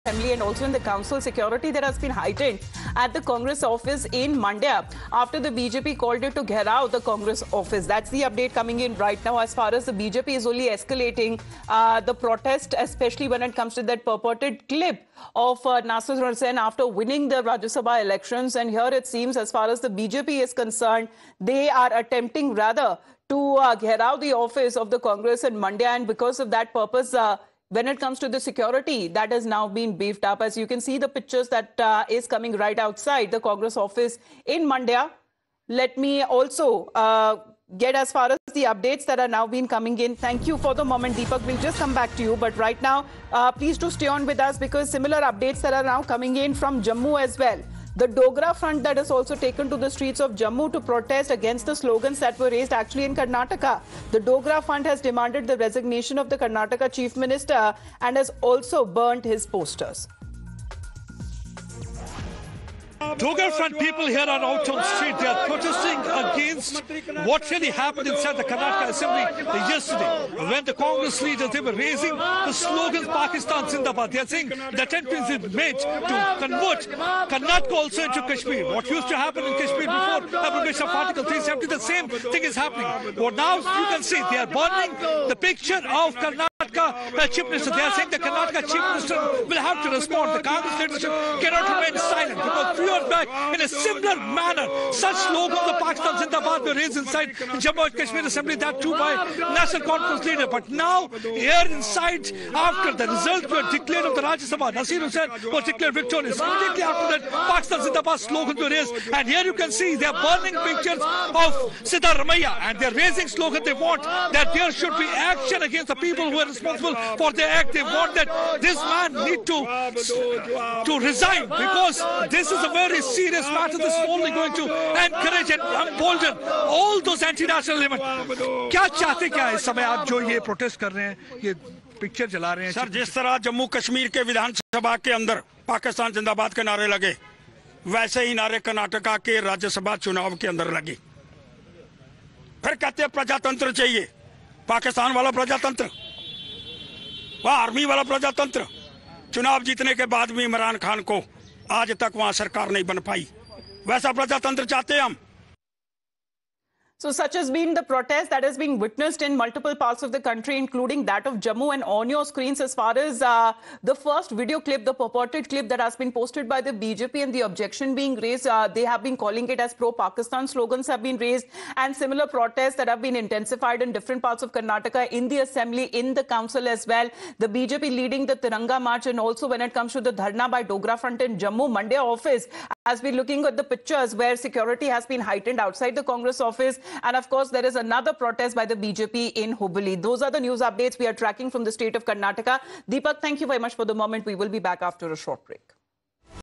'Pakistan Zindabad' Slogan Raised In Karnataka, Dogra Front Stages Protest I Times Now
BJP alleges that 'Pro-Pakistan' slogans are raised in Karnataka after in the Karnataka Vidhan Sabha, in support of the Rajya Sabha Member Syed Naseer Hussain. Whereas, FIR has been filed for the incident that took place at Karnataka Vidhan Sabha.